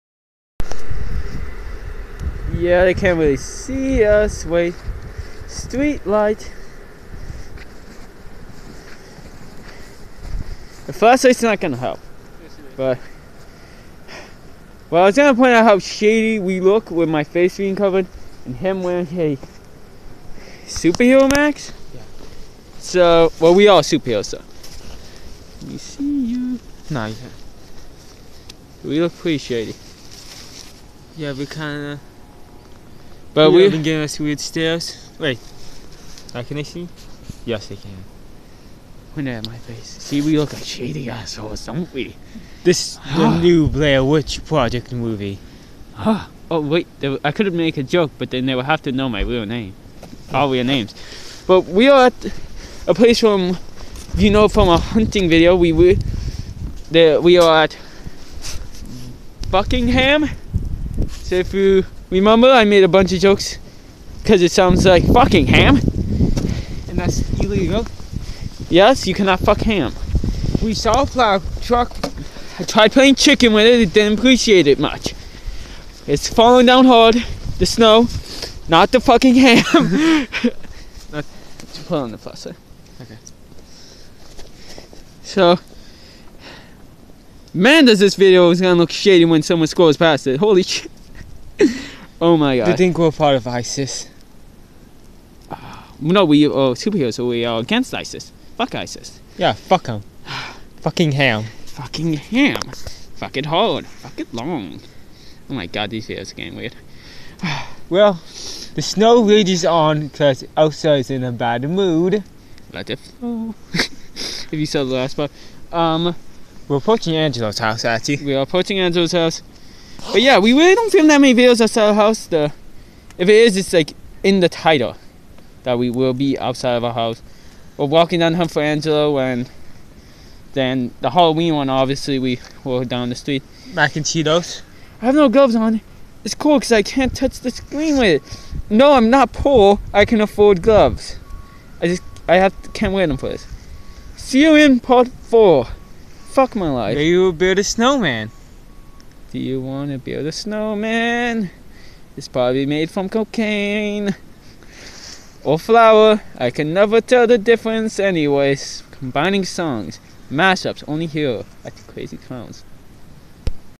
The Adventure Into The Snow pt. 3
Birdman and The Raven visit Buckingham and make jokes about the Blair Witch Project